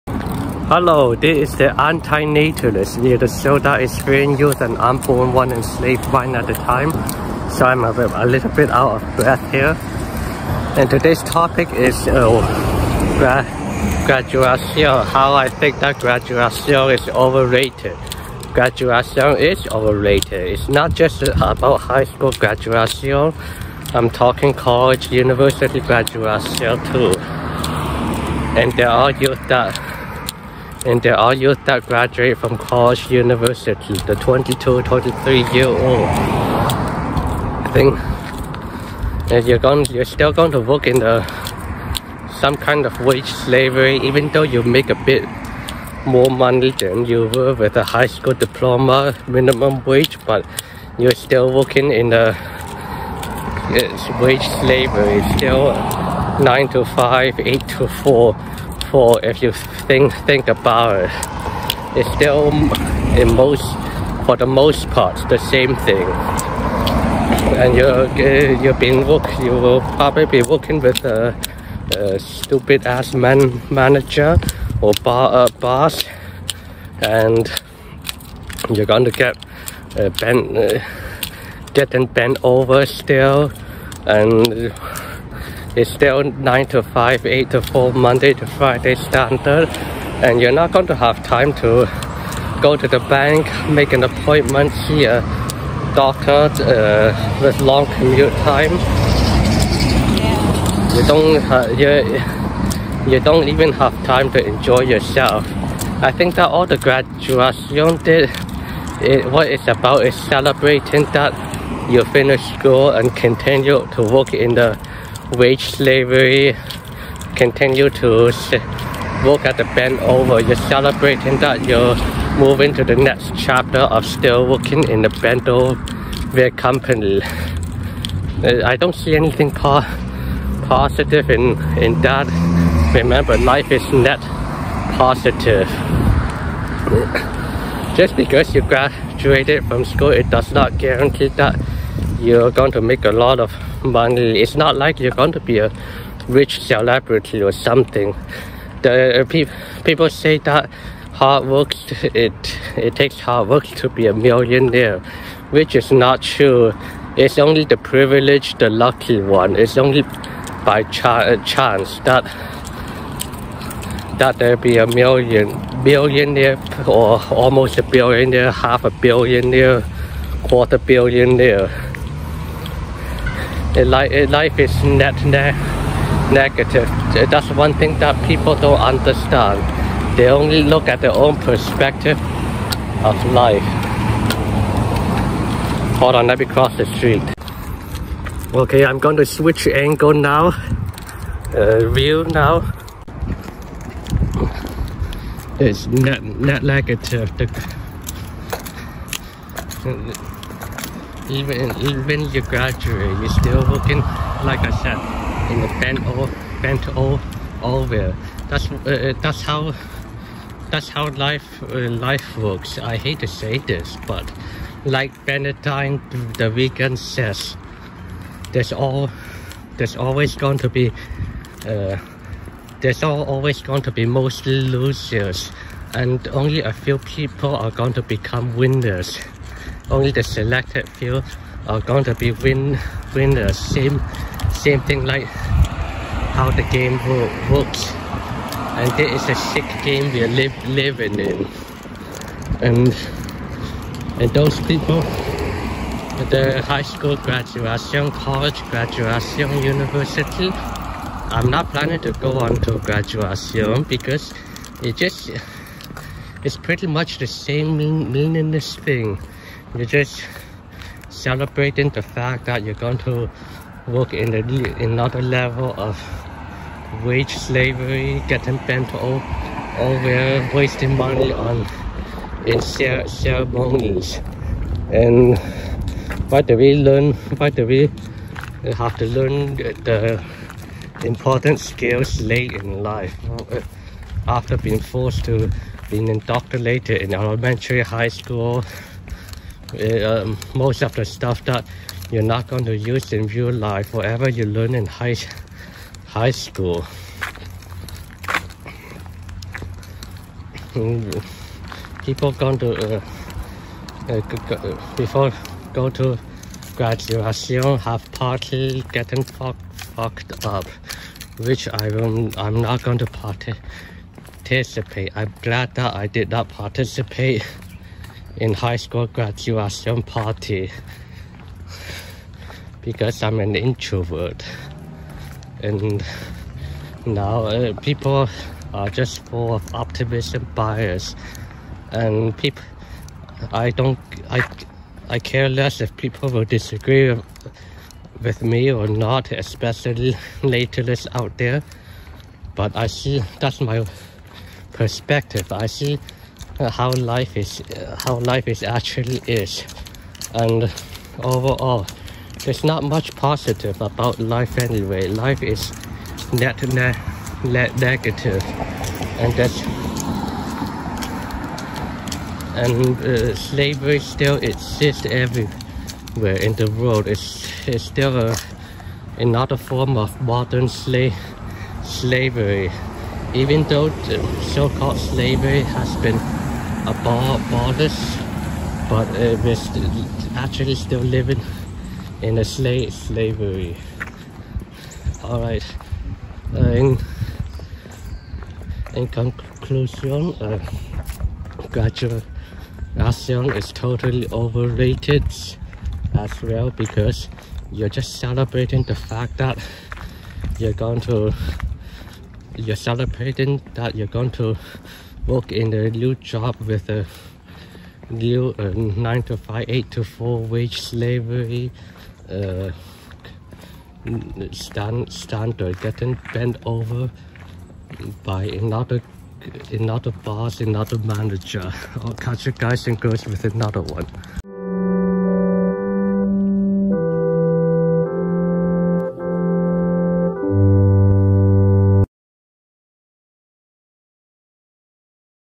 Graduation Celebration is overrated
Graduation is celebrating the fact you are about to enter the same old 9 to 5 wage slavery, getting bentover and become the company simp. The another form of wage slavery. Not much change after graduation